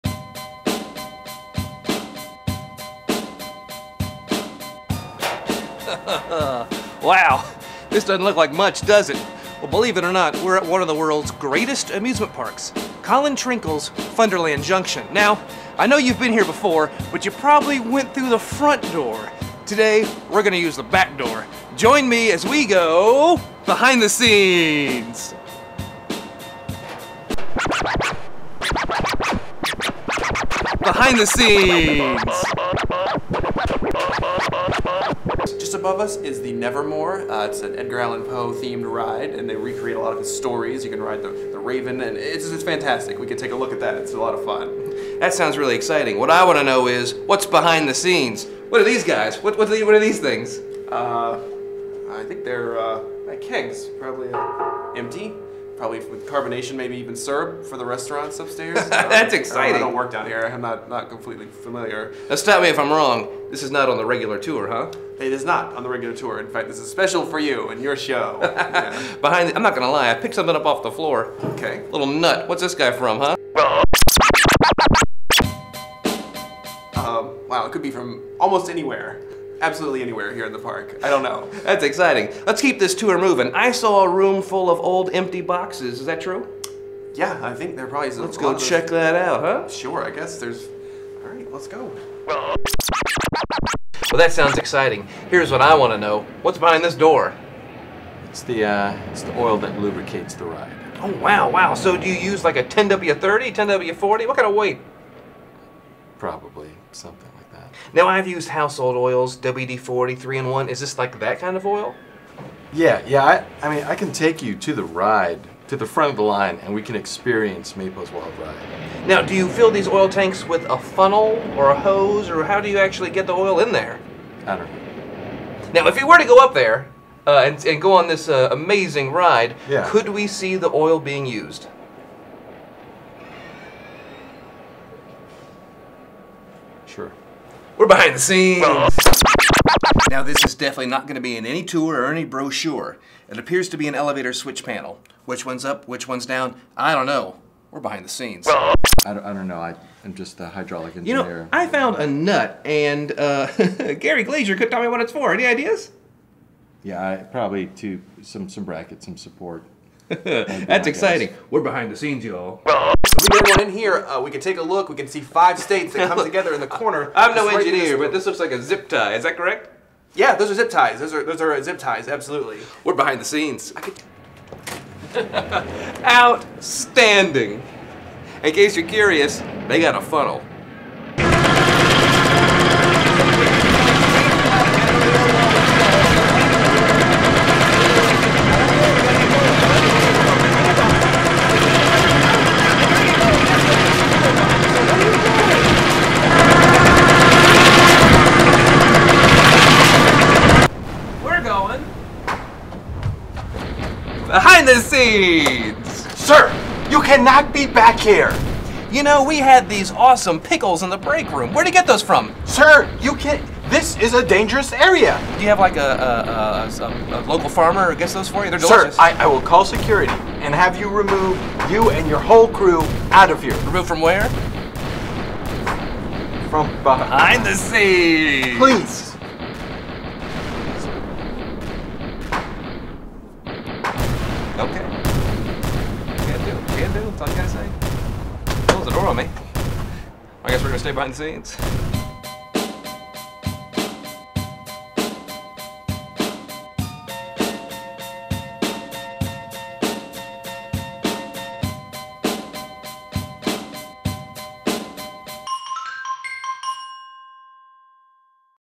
Amusement Park
Guy Summers always goes through the back door. Join him as he shows you what happens behind the scenes of the most exciting locations on the planet.